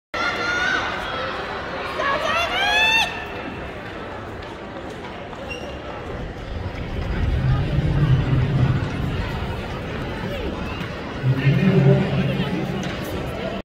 David West 60m 11-12 Boys USATF 8.77 The Bama Showdown January 30 2022
David West
60m
11-12 Boys
USATF
8.77
The Bama Showdown
January 30 2022
SBR Jaguars